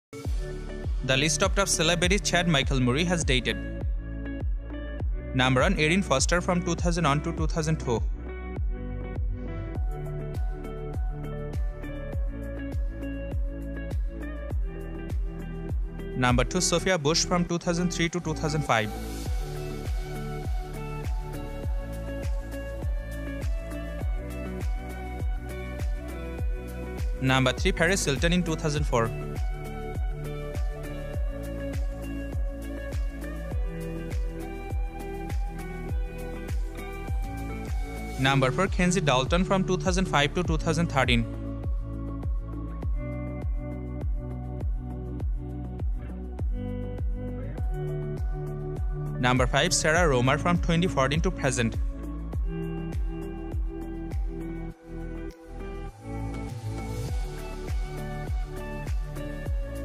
Chad Michael Murray ★ Girlfriend ★ Sarah Roemer ★ Kenzie Dalton
The Abundant World Presents, Chad Michael Murray ★ Girlfriend ★ Sarah Roemer ★ Kenzie Dalton ChadMichaelMurrayShorts.

Born on August 24, 1981, Chad Michael Murray is an American actor, writer, and former model. In addition to recurrent appearances on the same network as Tristin DuGray on Gilmore Girls (2000–01), Charlie Todd on Dawson's Creek (2001–02), and Edgar Evernever on Riverdale (2019), he portrayed the principal character of Lucas Scott in The WB/CW adolescent drama series One Tree Hill (2003–09, 2012).